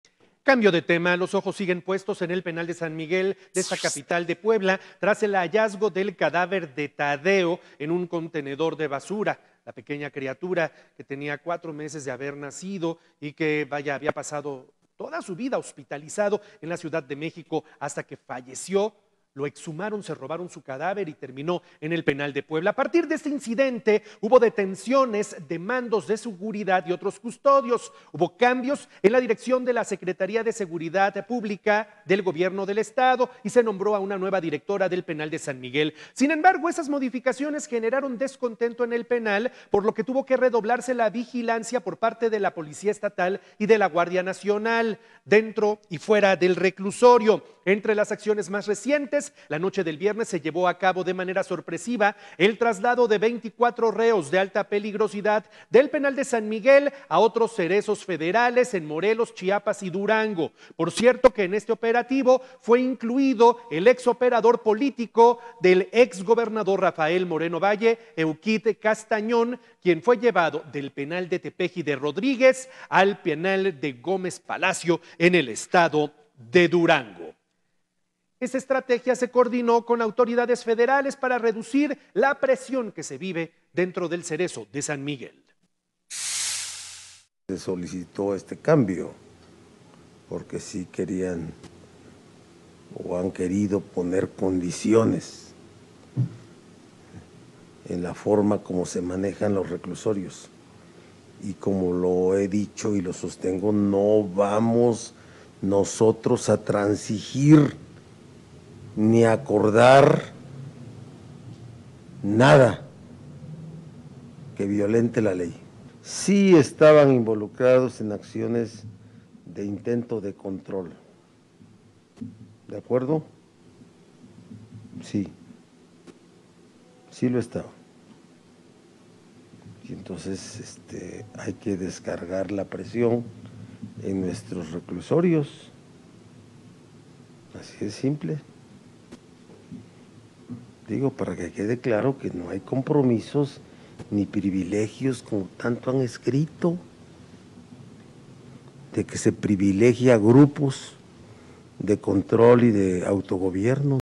Traslado de reos || Noticias con Juan Carlos Valerio
Trasladan a reos de alta peligrosidad del penal de San Miguel a cárceles federales de diferentes partes del país, en este operativo fue incluido el operador político del ex gobernador Rafael Moreno Valle, Eukid N.